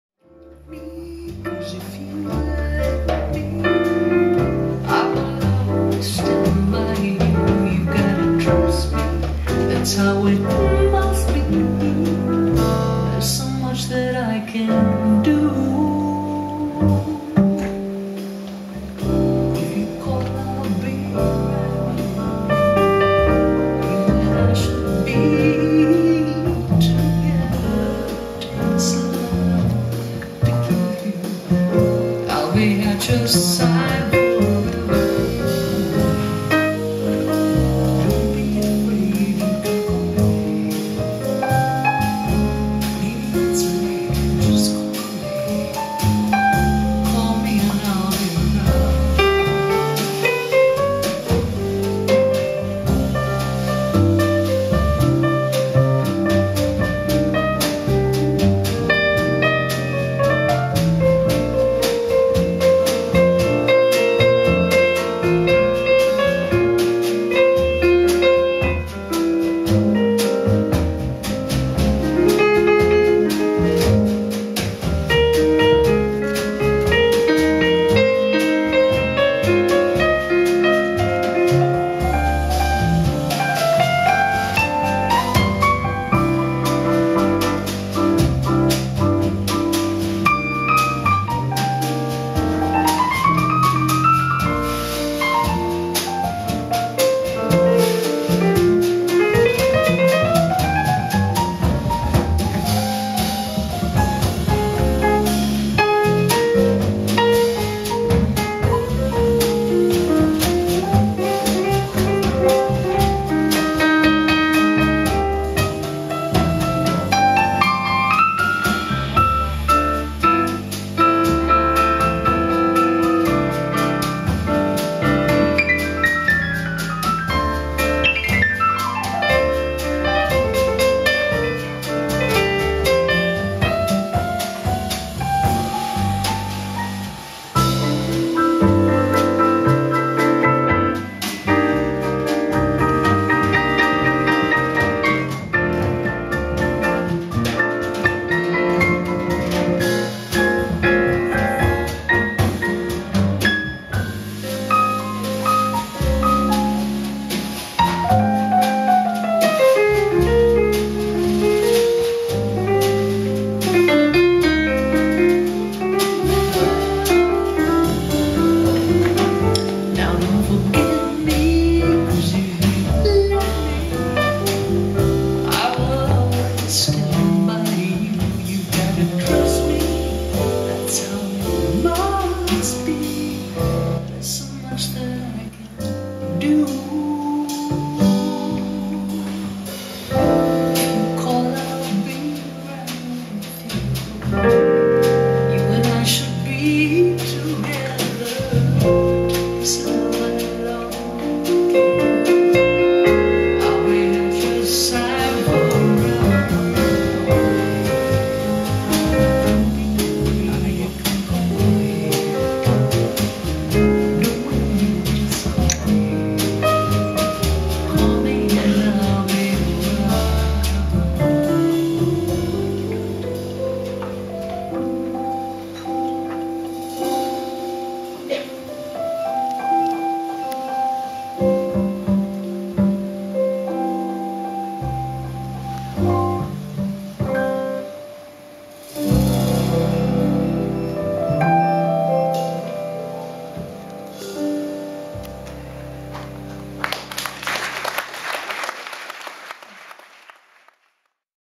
Patricia Barber - NattjazzLive
Røkeriet USF:23.mai 12 - PATRICIA BARBER piano/vocal at "Nattjazz"  Bergen Jazzfestival - LARRY KOMUT bass - JON DEITEMEYER drums -